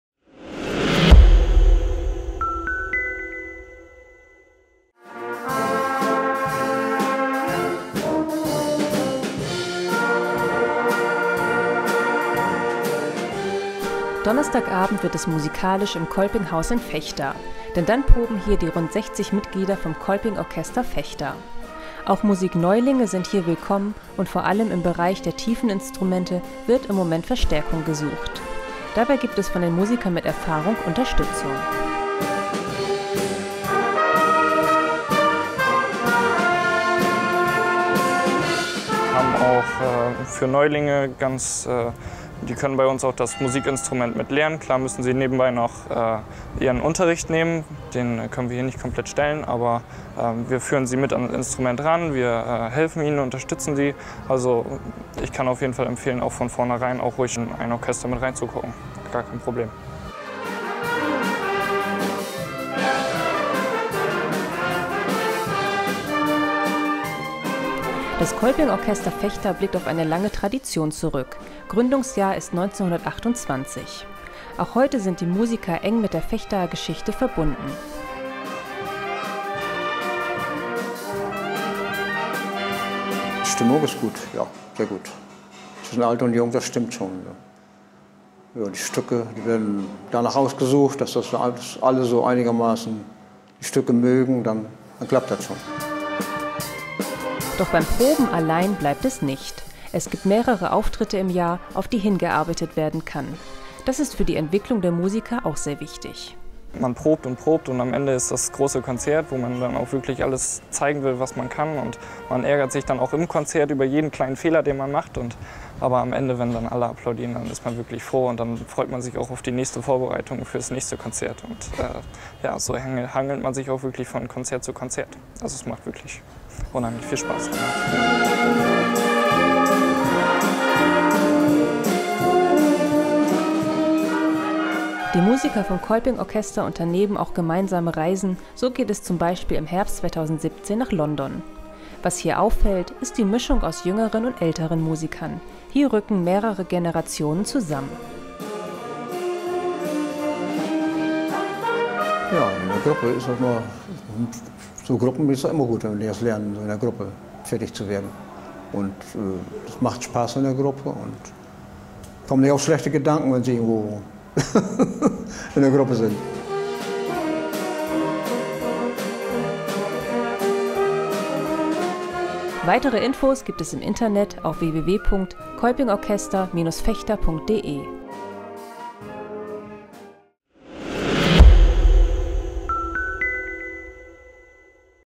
Musikserie Teil 14: Kolpingorchester Vechta
„Wir machen Musik“ Teil 14: Dieses Mal stellen wir auch das Kolpingorchester aus Vechta in unserer Musikserie vor. Weitere Infos zum Thema gibt es in der gedruckten Ausgabe der OV am Dienstag (16.08.) und auch im epaper.